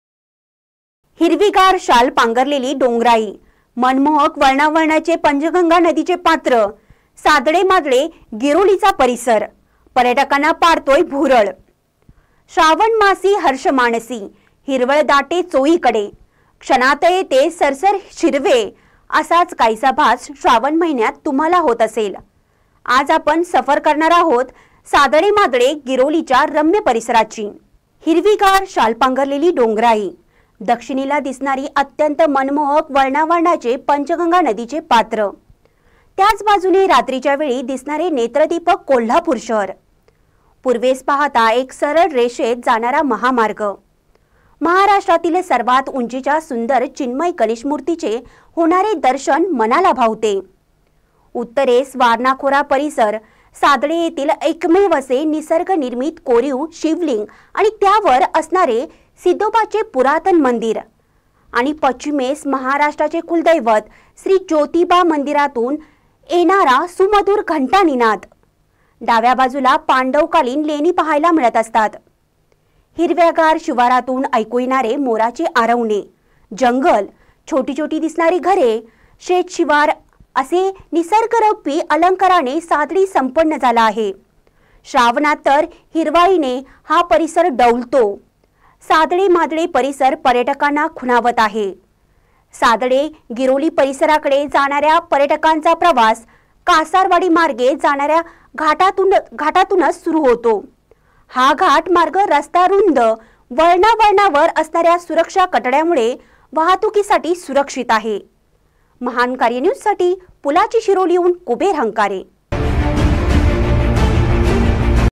हिरवीगार शाल पांगरलेली डोंगराई ..... मनमोहक वळणा वळणाचे पंचगंगा नदीचे
हिरवीगार शाल पांगरलेली डोंगराई मनमोहक वळणा वळणाचे पंचगंगा नदीचे पात्र सादळे,मादळे , गिरोलीचा परिसर पर्यटकांना पाडतोय भुरळ